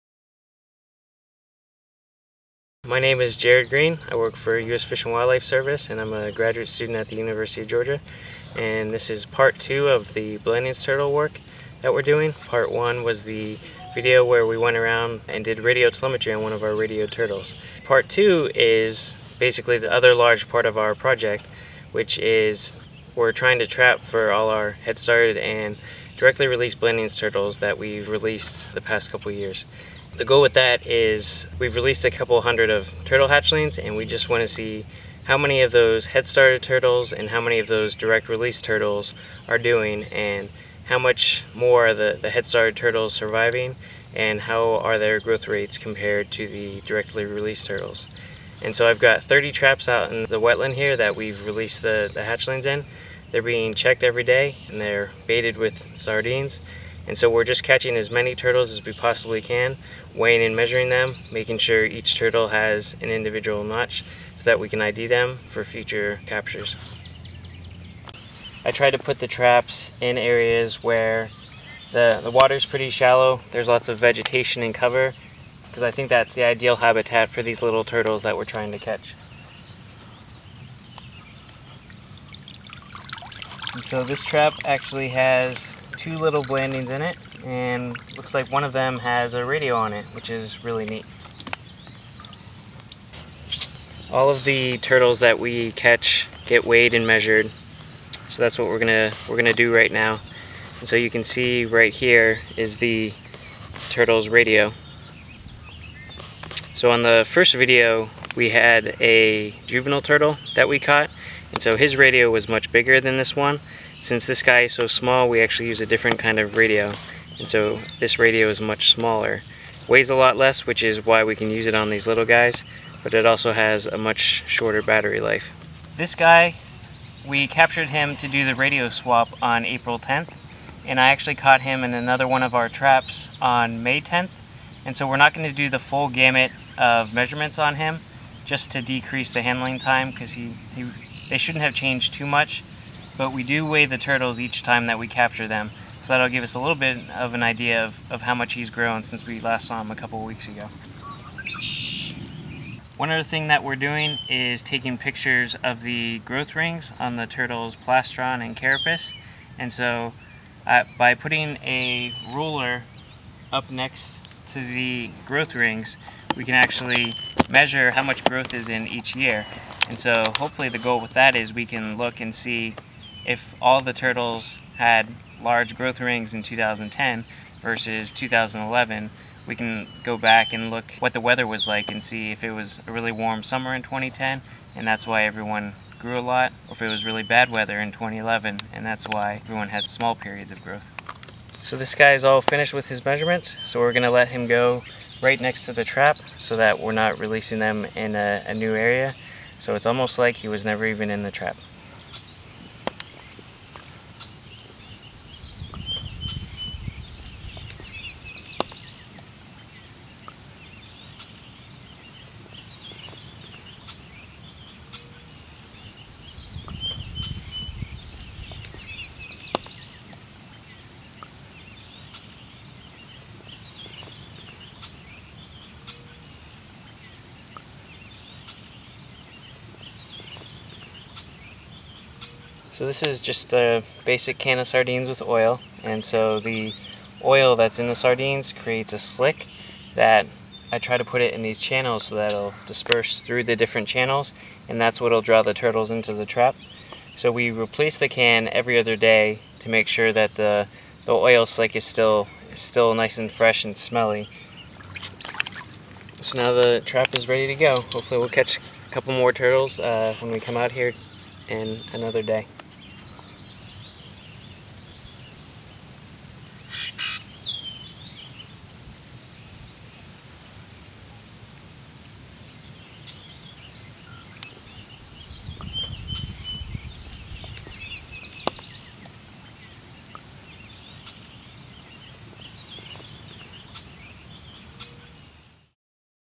Blanding's Turtle Research and Repatriation at Assabet River NWR (Part 2)
This is our second installment describing the research and repatriation of the Blanding's Turtle in the Eastern Massachusetts National Wildlife Refuge Complex.   In this video we see how the head-started and released turtles are regularly trapped and monitored to gauge their growth.

Please see our first video (available in different levels of detail which covers how we use radio tracking to monitor released turtles within the refuges.

Produced in cooperation with the U.S. Fish and Wildlife Service.